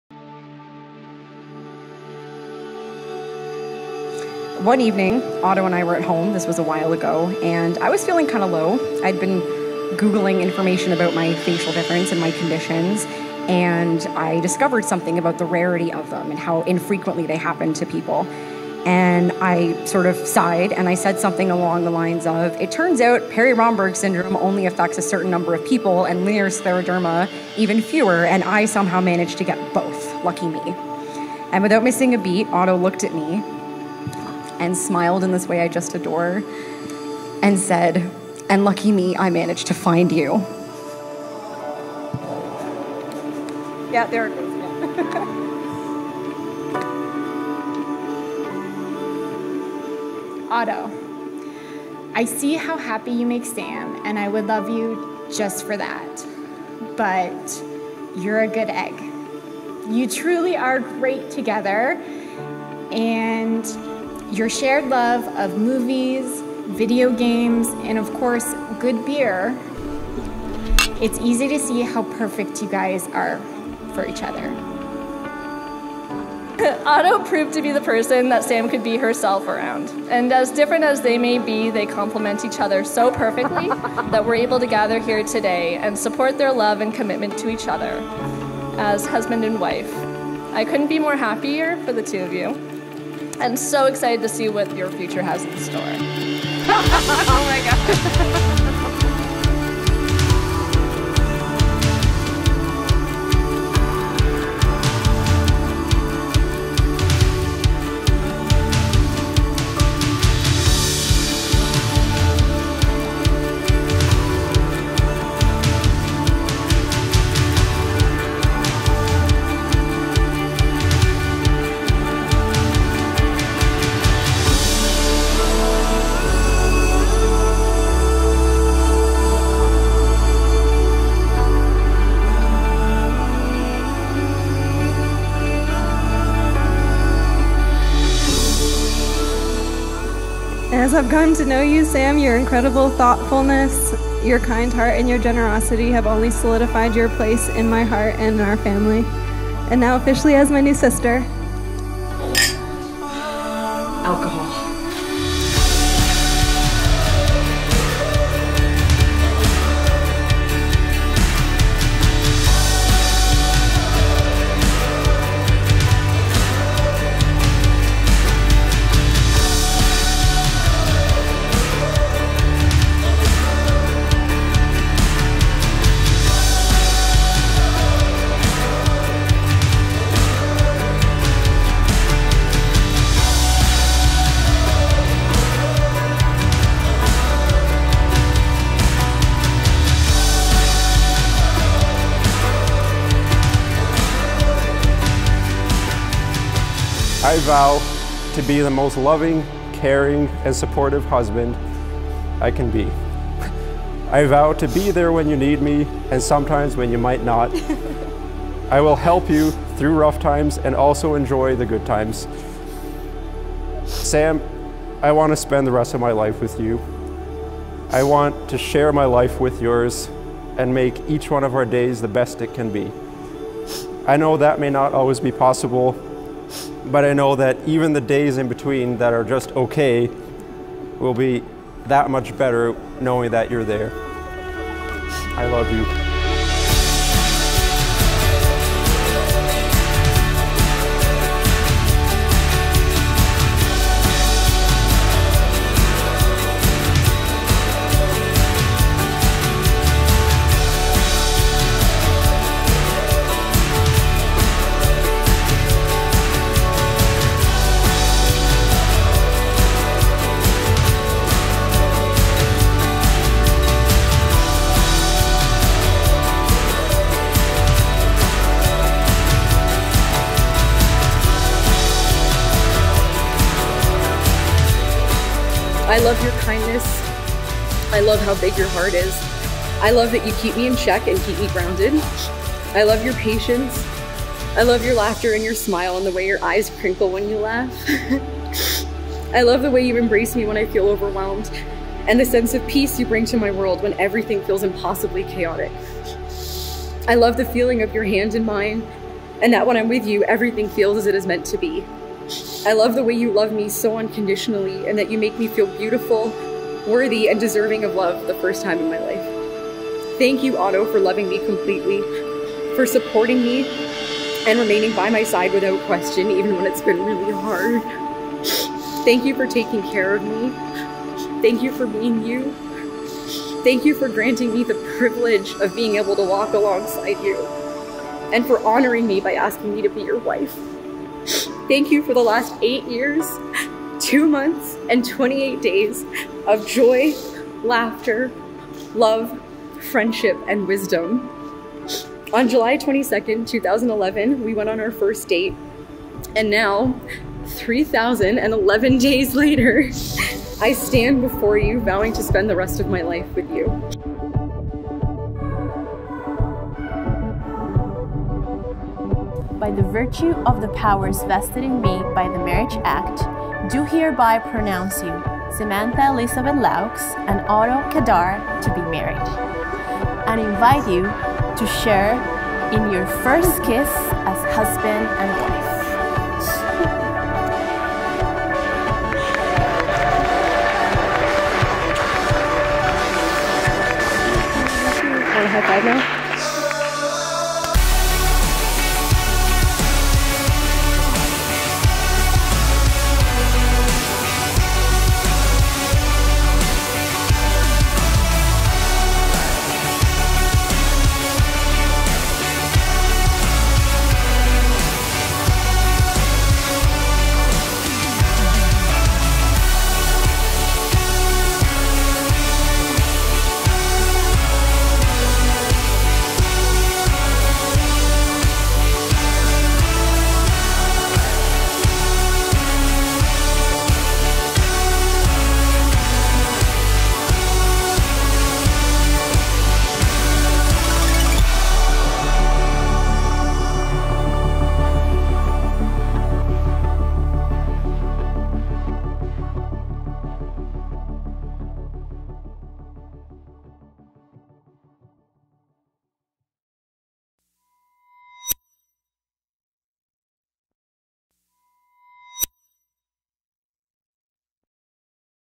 Grey Silo Wedding | Samantha + Otto
When you find your soulmate, you realize how lucky you are to meet them. To walk side by side, and hand in hand with them in marriage, that is love. Samantha and Otto were lucky to meet each other, I was lucky to have witnessed their love story. Thank you for letting us see and feel true love. Congratulations Sam and Otto.